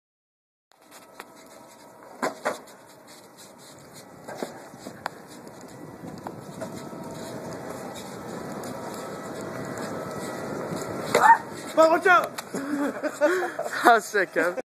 Kevin Carr at Penn State University
Kevin Carr on the Loaded Tan Tien with a 50 ft manual at Penn State University